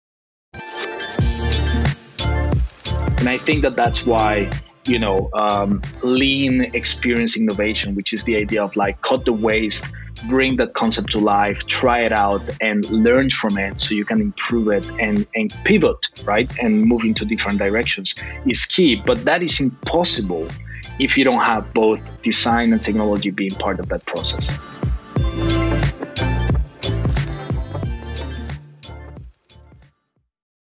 Ep.4 Teaser: Using AI and machine learning to reduce uncertainty
Hear Anthony, Executive Technology Director at R/GA, speak about the interplay between technology and design – explaining the impact that tools like AI, language processing, and machine learning can have on the consumer experience.

We partnered with Design Untangled for a series of interviews with design leaders on navigating today’s unprecedented conditions and challenges.